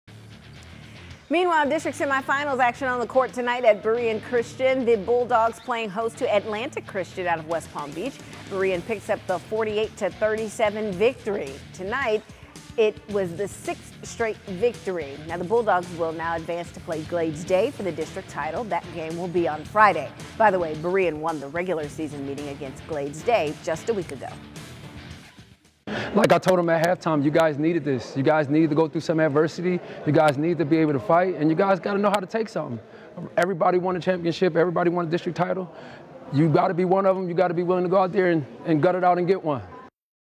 Berean Christian picks up playoff win
Berean Christian knocks off Atlantic Christian to head to the district title game.